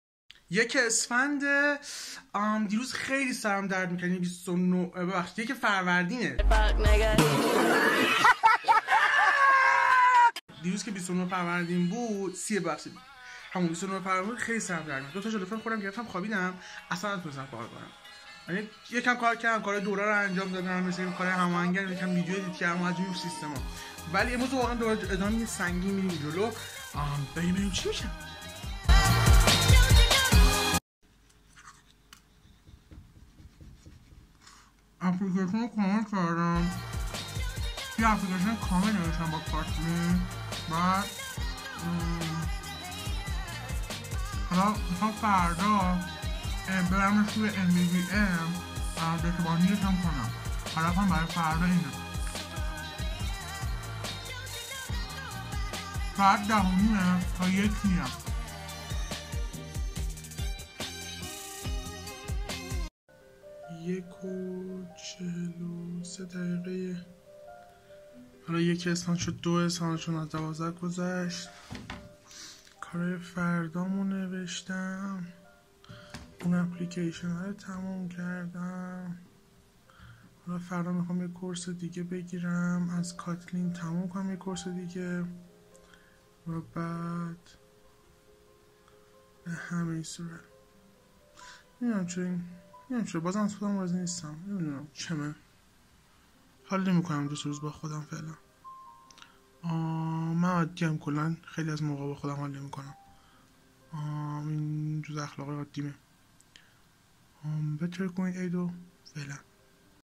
نوروز ولاگ ۲ : مجدد کاتلین با پروژه
یه پروژه با کاتلین اوردم بالا حالا میخوای روش ارچیتیکت و دیتابایندیگ پیاده سازی کنم